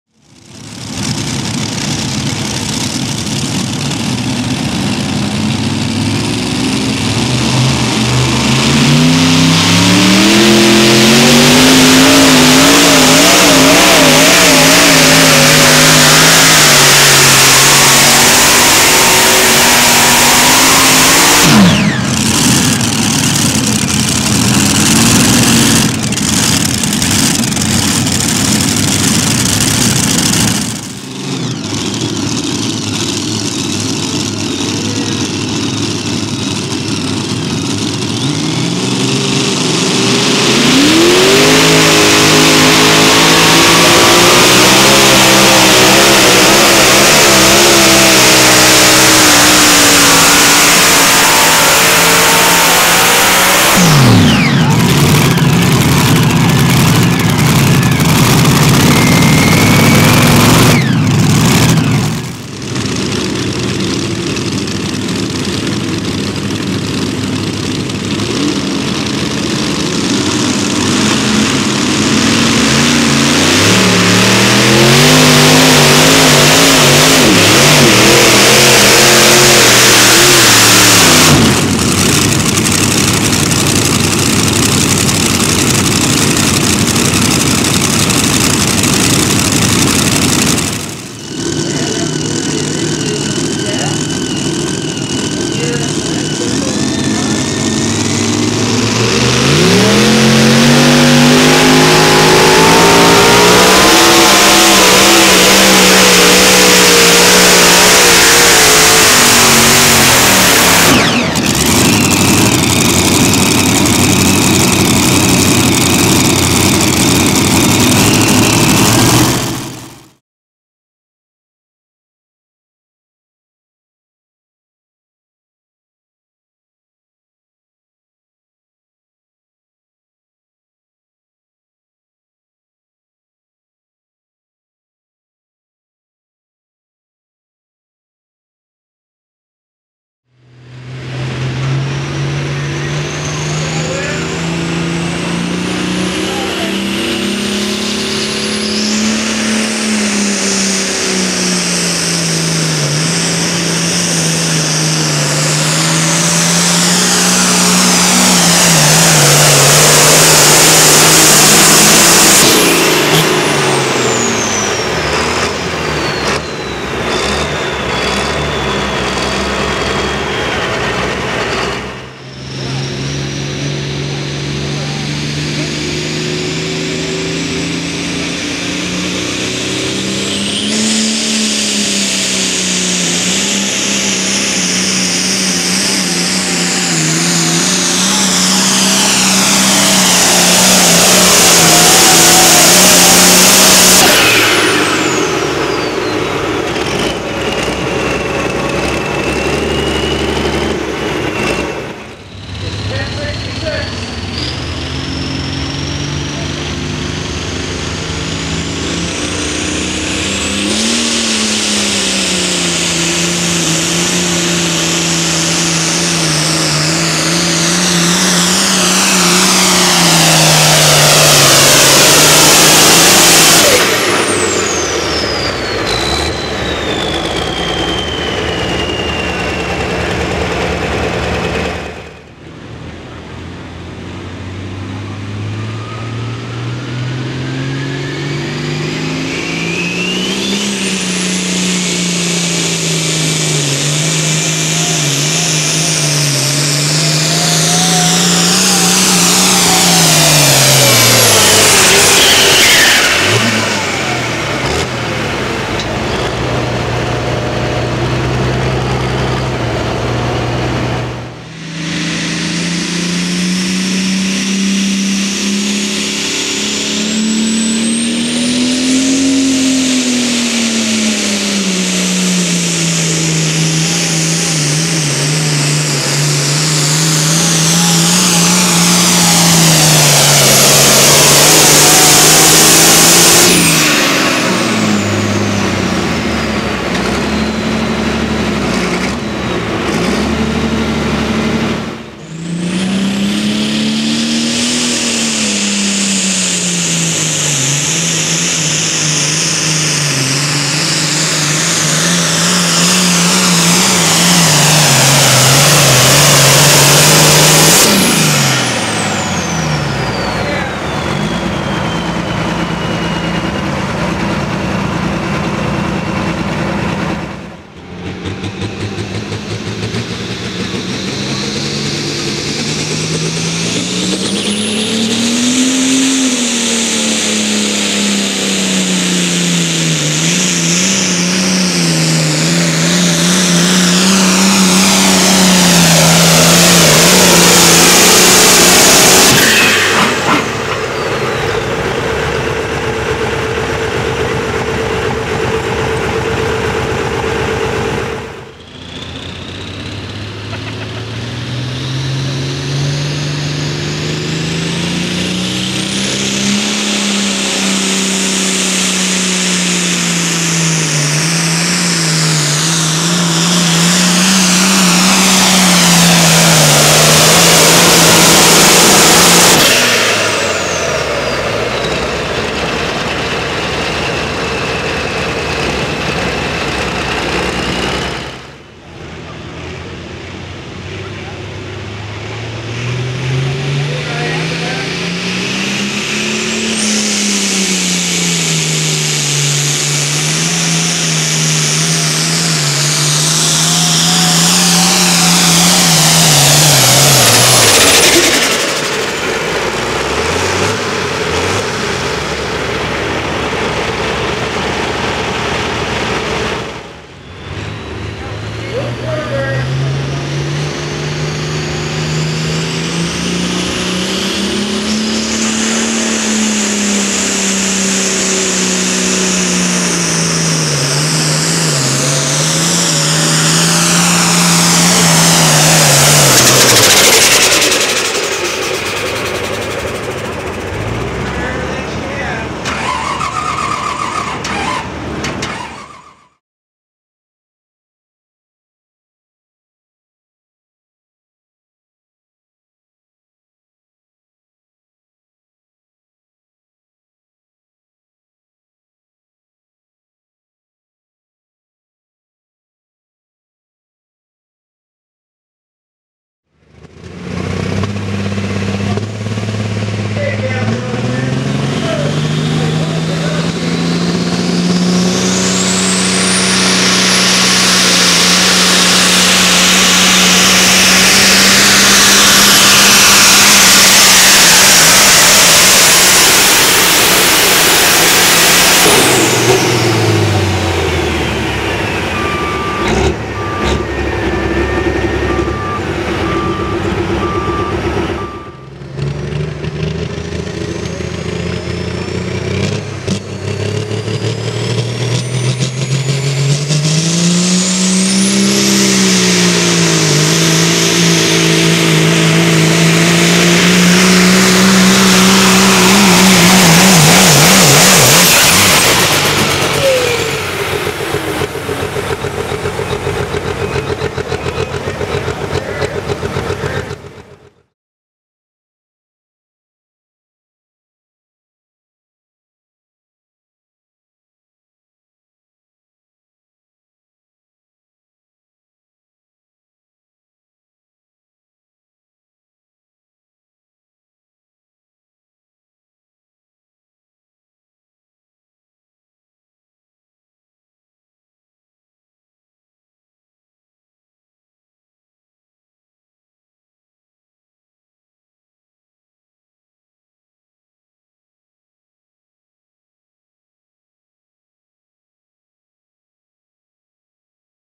Modified Tractors, Pro Farm Tractors & Pro Stock Semis Pulling in Austin, MN - Tractor Pulling 2022
Mower County Fair Pull
Mower County Fairgrounds
Austin, MN

Modified Tractors, Pro Farm Tractors & Pro Stock Semis pulling at the 2022 Mower County Fair Pull in Austin, MN